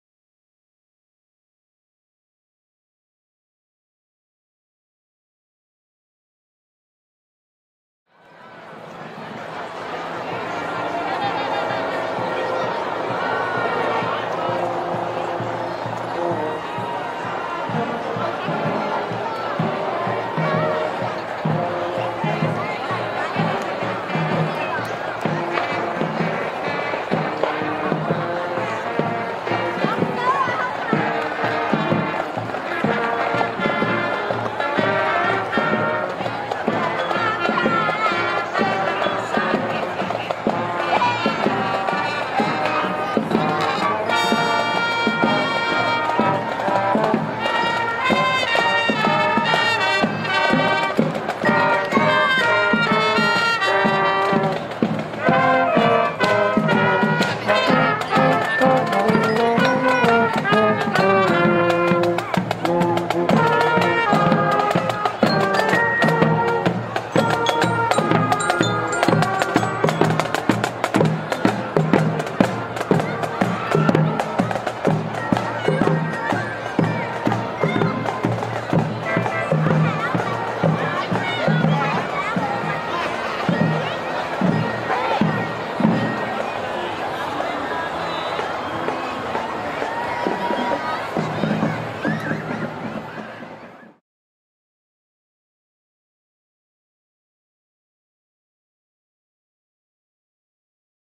Bluestone High School Marching Band in 2021 Raleigh Christmas Parade
The Bluestone High School (Skipwith, VA) Marching Band in the 2021 Raleigh, North Carolina Christmas Parade 11/20/2021.  This video was taken at the north end of Fayetteville Street on street level this year due to temporary closure of the parking garage.